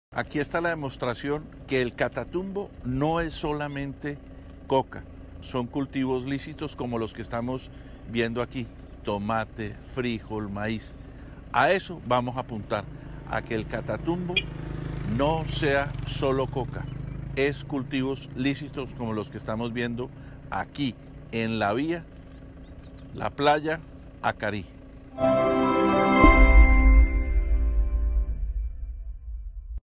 “El Catatumbo no es solo coca”: Carlos Negret, Defensor del Pueblo.
Declaraciones del Defensor del Pueblo durante el recorrido que adelanta por la región del Catatumbo.